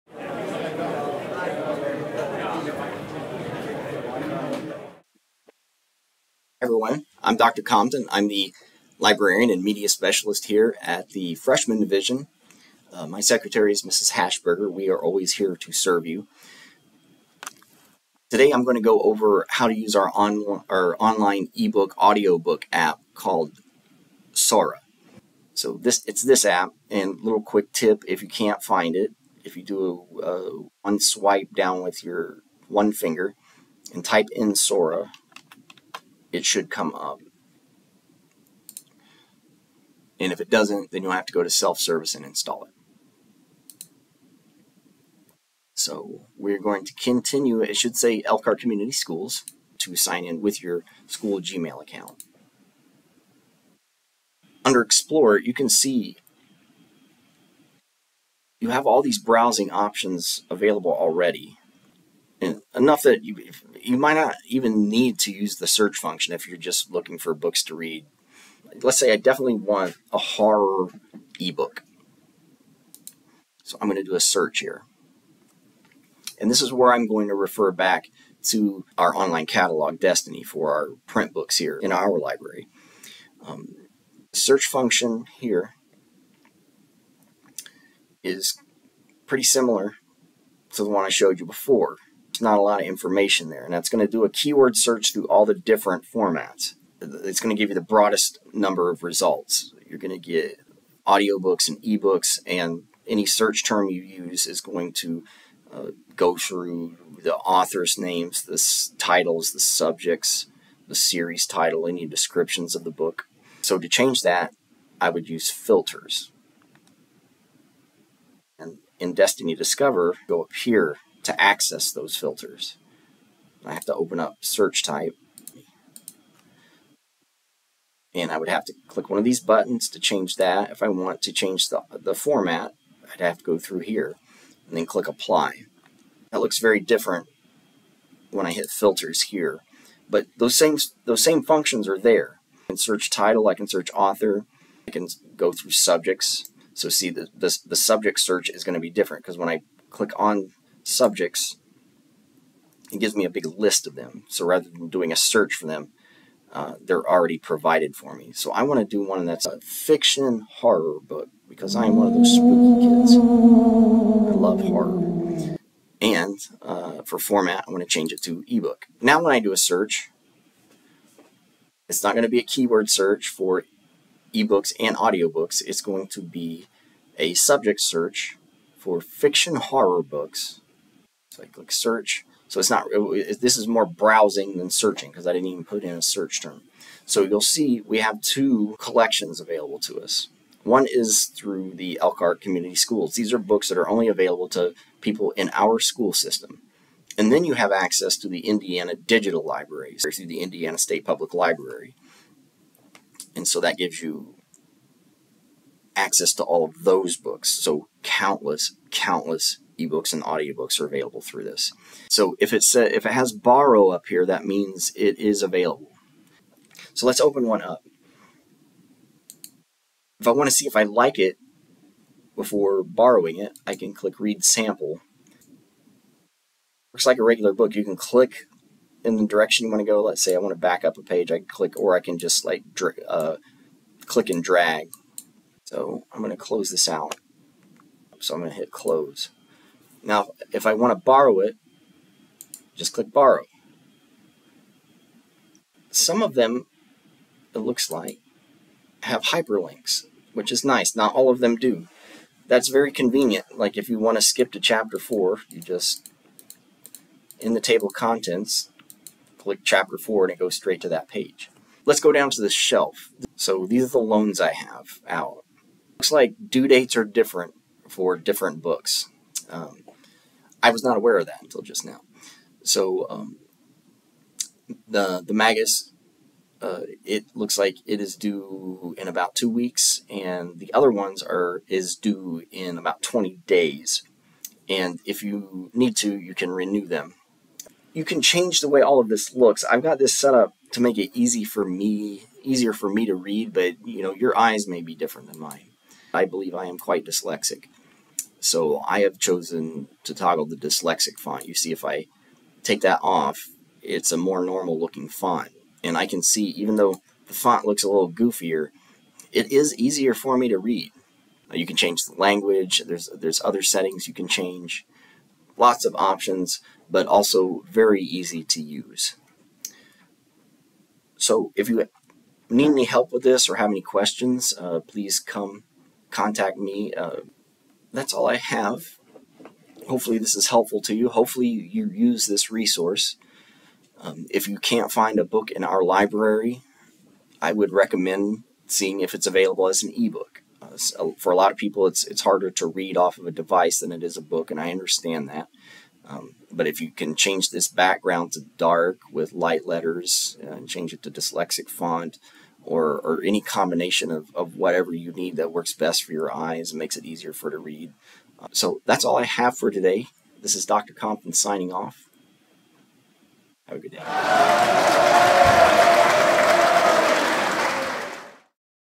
SORA eBook & Audiobook Lesson 2023
This tutorial guides Elkhart Freshman Division students through accessing and using the SORA eBook/Audiobook app installed on their school iPads.